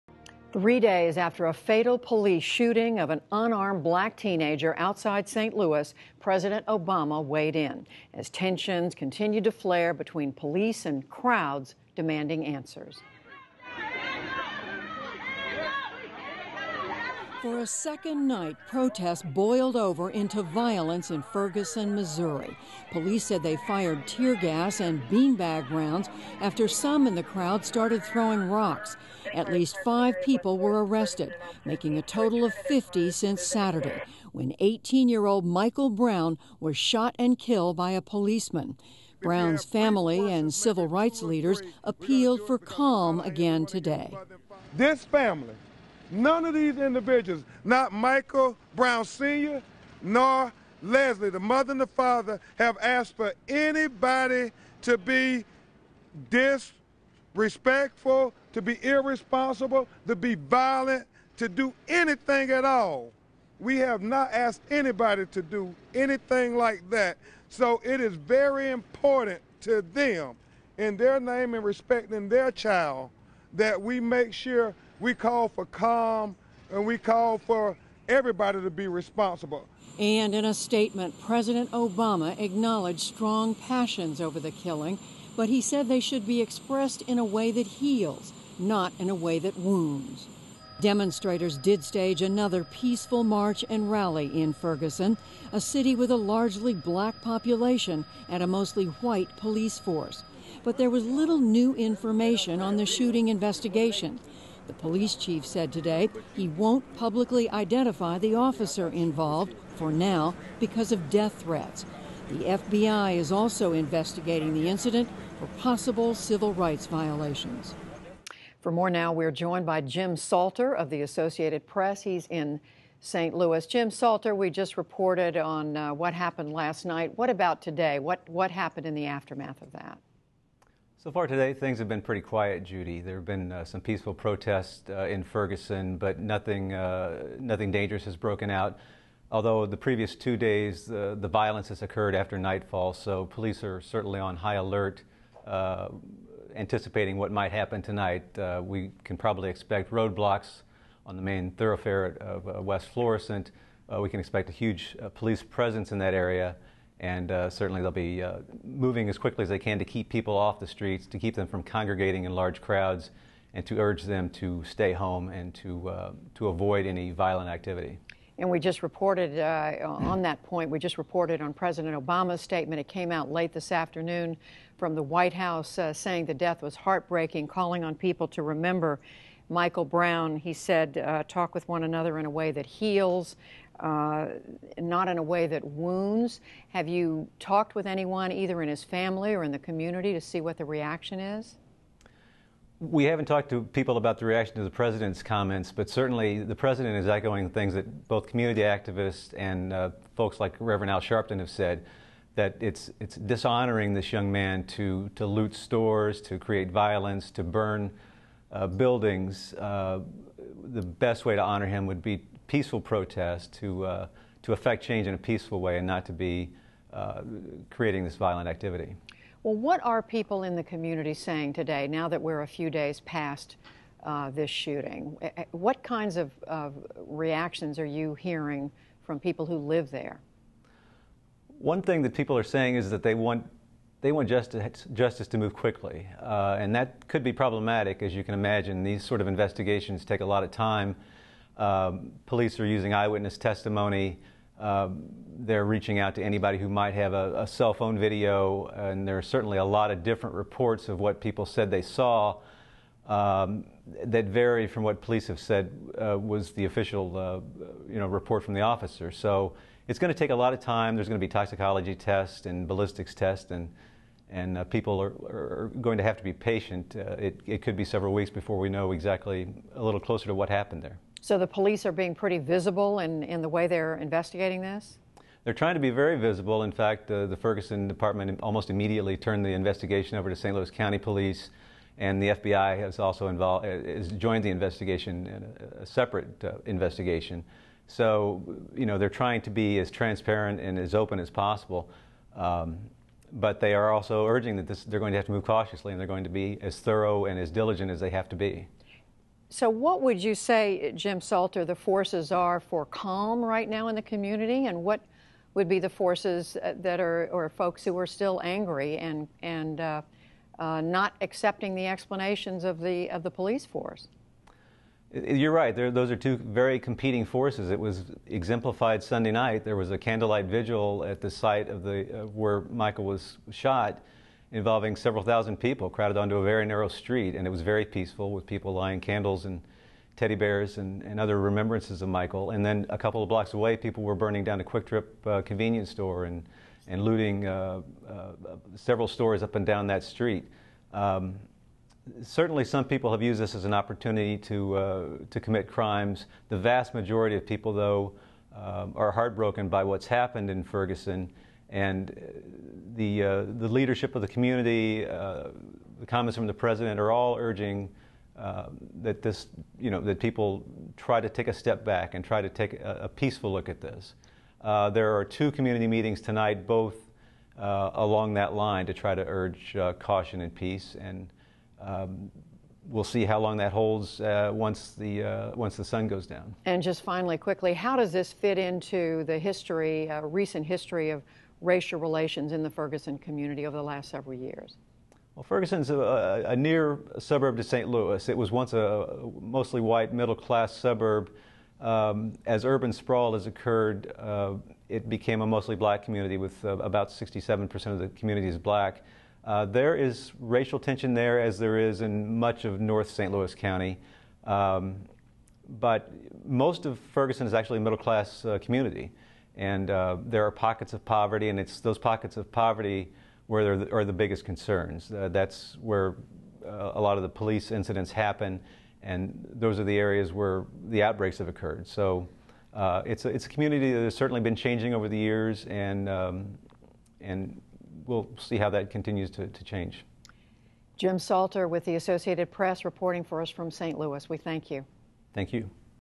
Family of slain Mo. teen call for calm after violent protest
For a second night, protests boiled over into violence in Ferguson, Missouri, after the fatal police shooting of an unarmed African-American teenager on Saturday. In a statement, President Obama said pain over the killing should be expressed in a way “that heals, not in a way that wounds.” For a closer look at the unrest, Judy Woodruff talks to Jim Salter of the Associated Press.